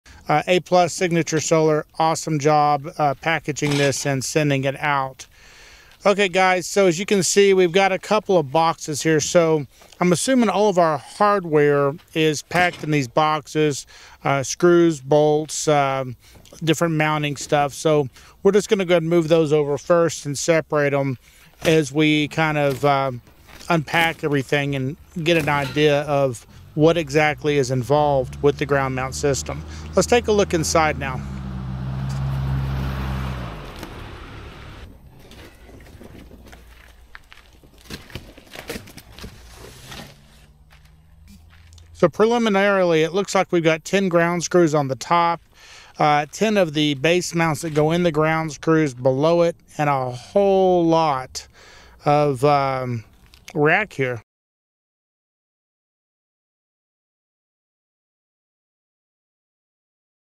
Signature Solar Review: Unboxing & Ground Mount System Hardware!
Unboxing the key components for a ground mount system, the anticipation builds as essential hardware is revealed. Will it all come together as planned, or will unforeseen challenges arise?